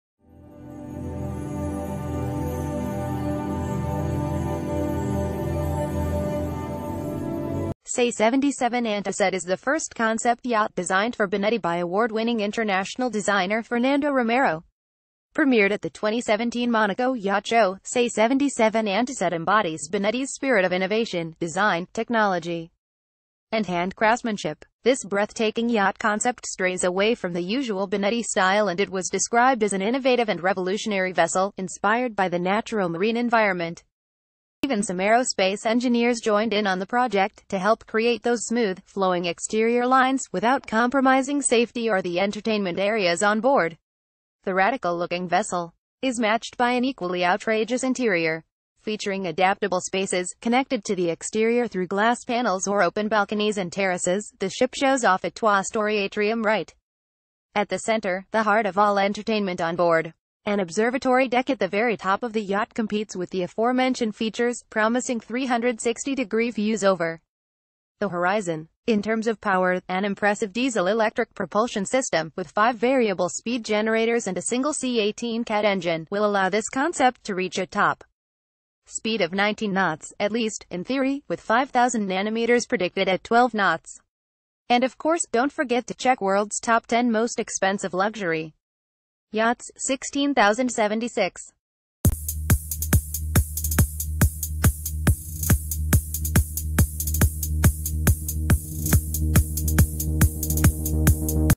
Breathtaking se77antasette luxury yacht by fernando romero
Se77antasette is the first concept yacht designed for Benetti by awardwinning international designer Fernando Romero. Premiered at the 2017 Monaco Yac...